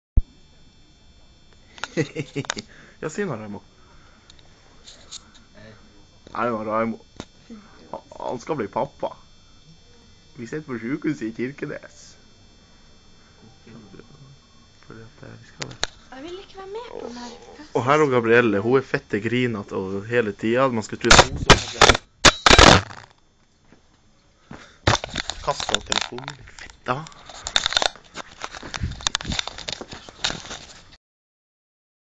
Raimo er snart pappa
Raimo, Eirik og gabrielle sitt på sykehuse, å gabrielle blir litt sur.